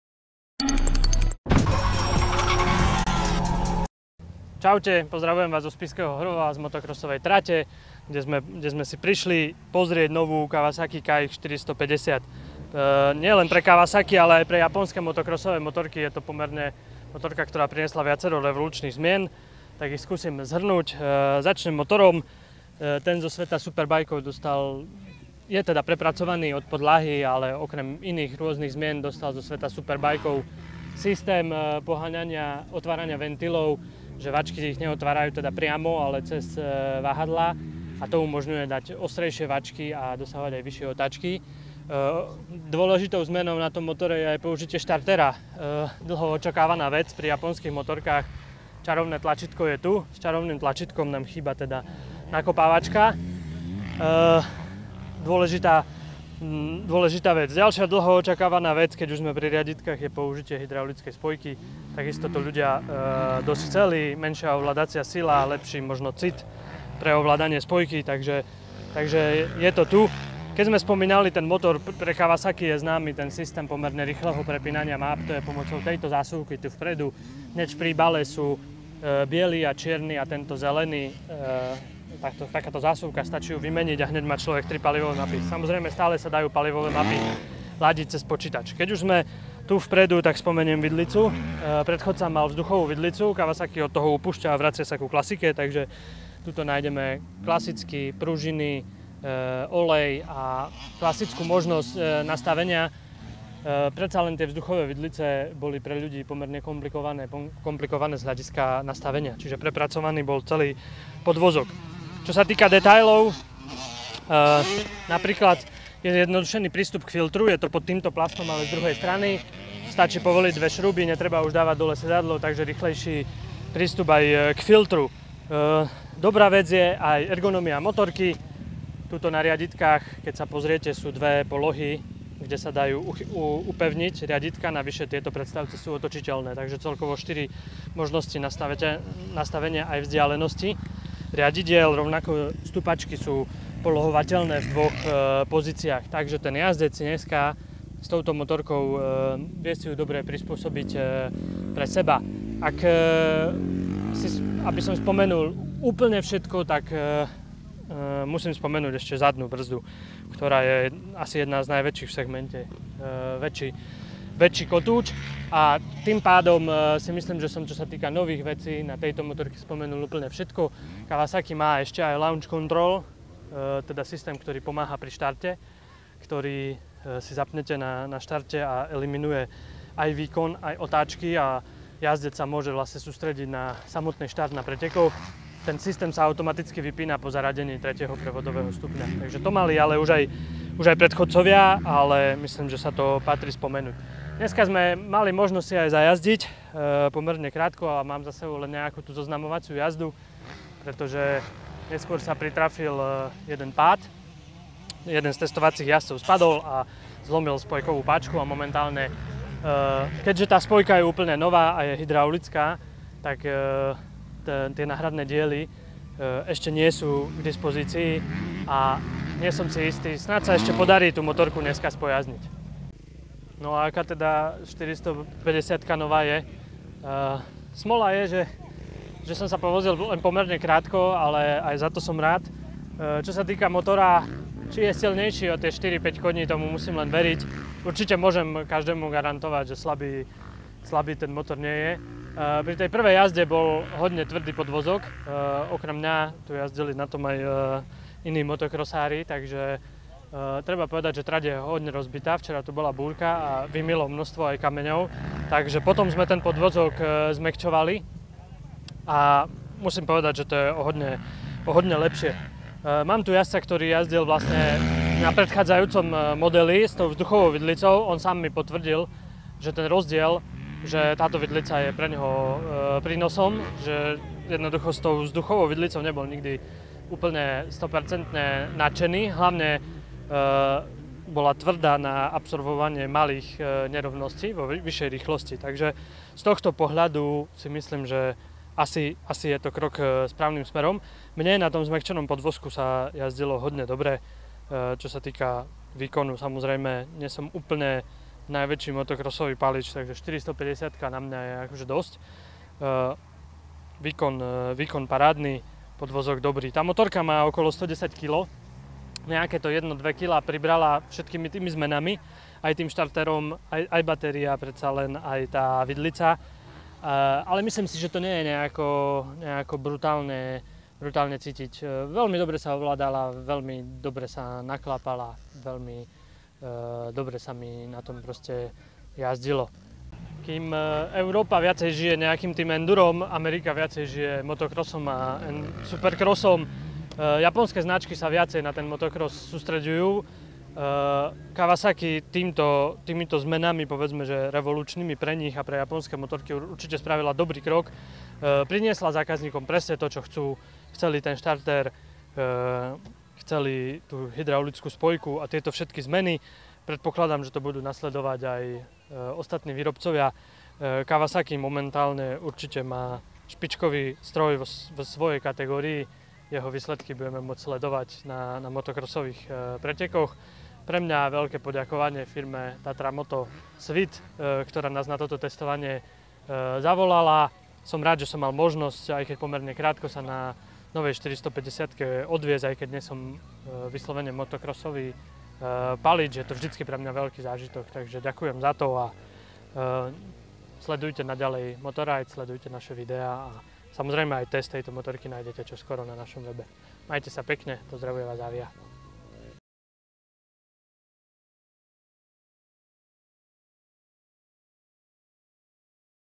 Kawasaki KX 450 2019 - MX revolúcia v japonsku - motoride.sk
Vo svete motokrosu sa inovuje a pre rok 2019 to Kawa nakopla naozaj významne, priaznivci motokrosu sa dočkali el. štartéra, hydraulickej spojky či návratu ku klasickej vidlici. Ale to zďaleka nie je všetko čo je nové...